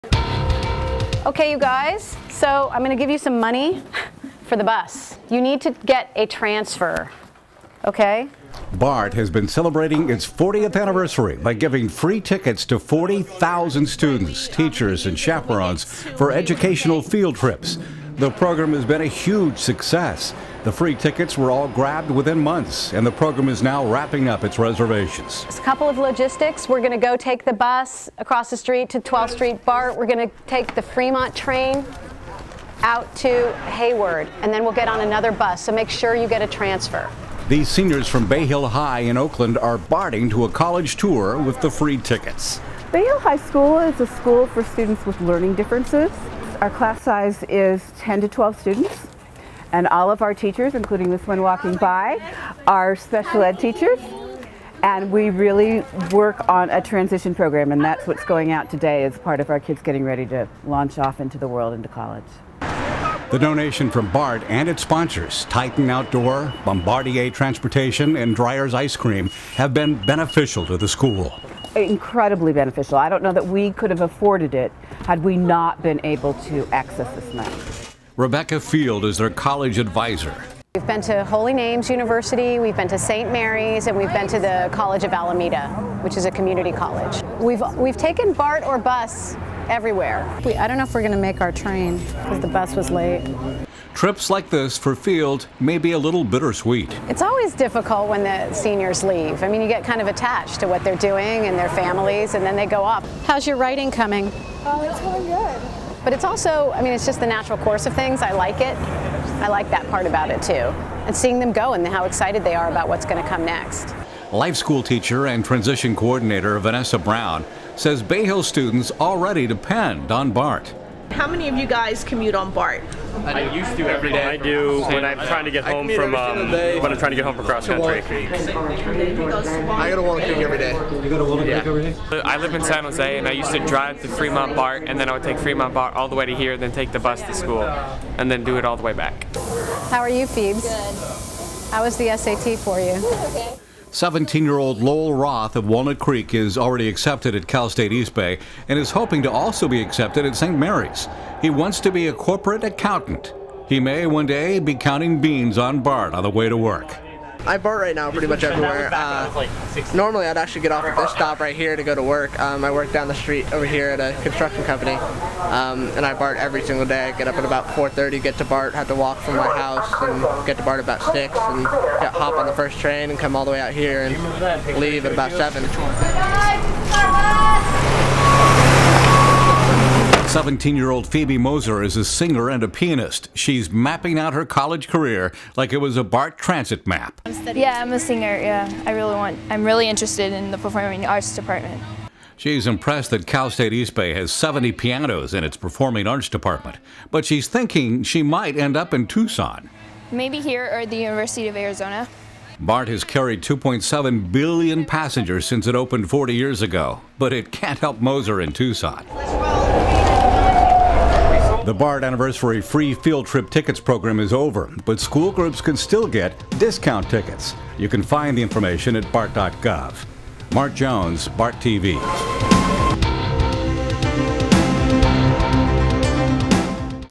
BART Anniversary Tickets
As part of its 40th Anniversary, BART gave away 40,000 free tickets to teachers and students for educational field trips.  They were the hottest tickets around.
Schools quickly grabbed the tickets and now the program is over, with some schools, like Bayhill High in Oakland, now using their reserved free tickets.